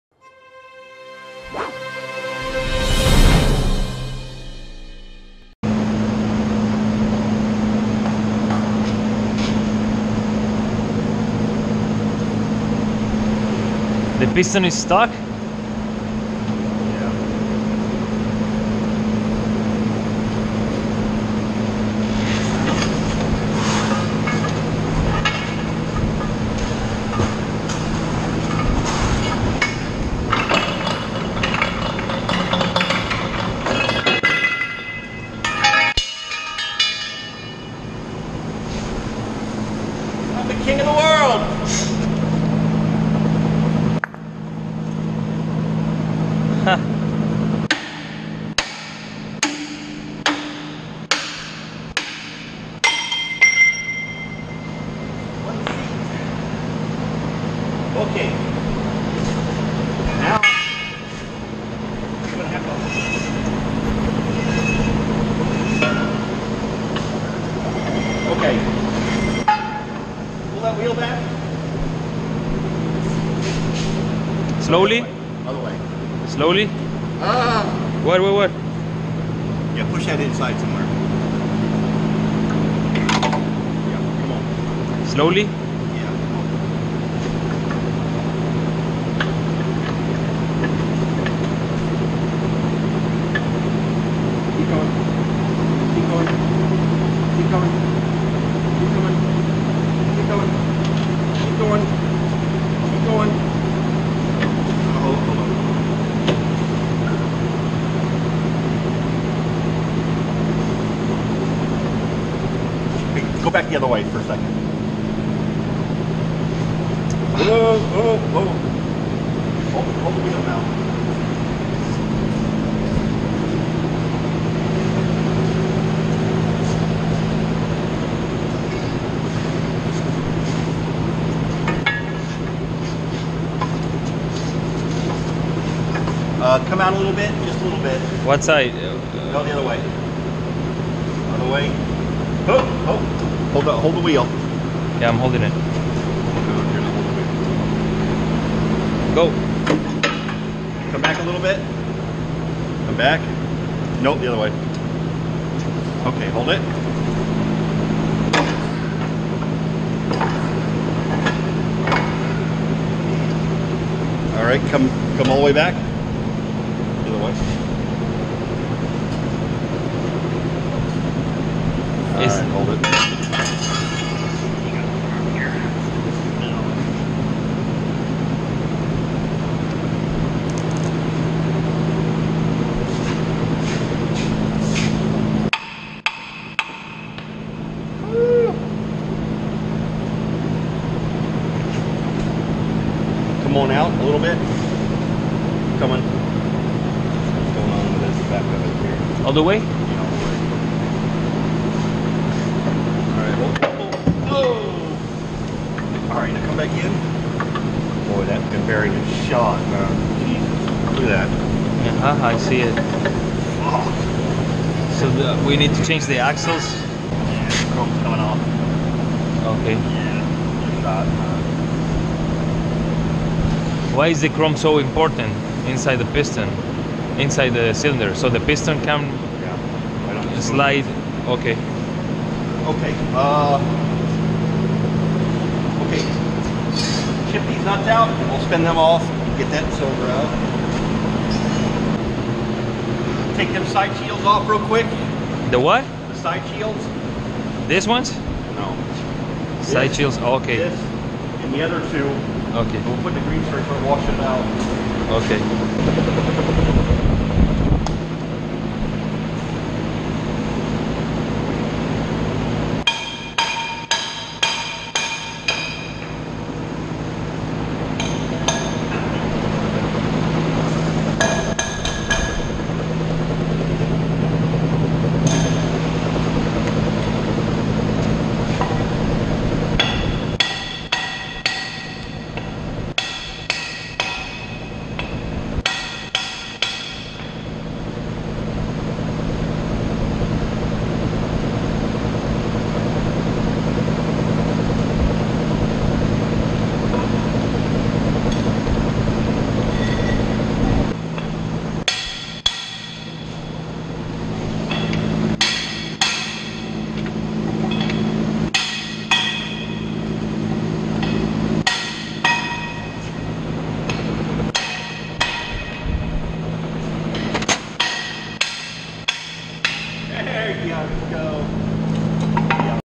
How To Disassemble Schwing P88
In this video we will demonstrate how to disassemble a concrete pump Schwing P88

JED Alliance Group has a large inventory of used concrete mixer trucks and concrete pumps for sale.